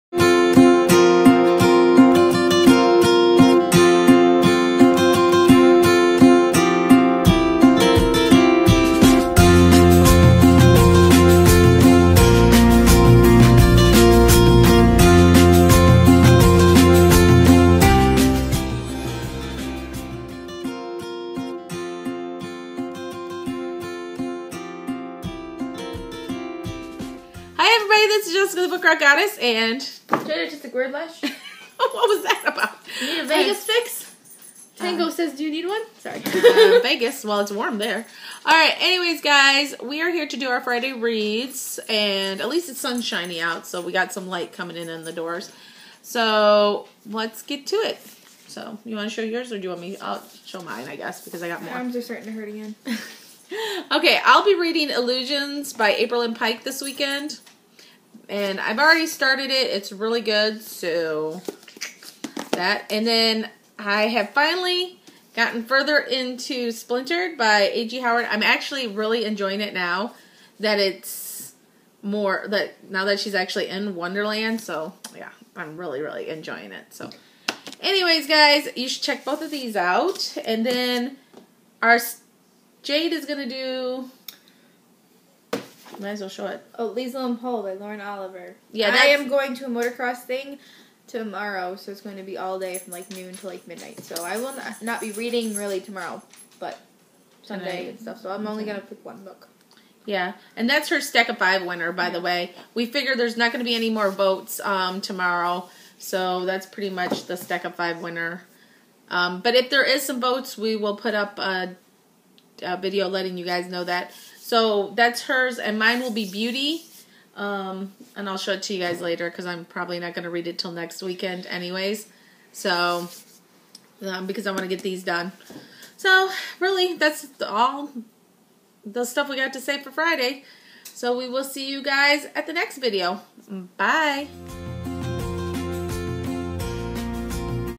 Fridayreads for 3/14/14 & possibly Stack of Five Winner
This is what we plan on reading for weekend.  Also Jayde will be gone all day tomorrow so we are posting stack of five winner early!
You can still vote but we are going with these until we see new votes.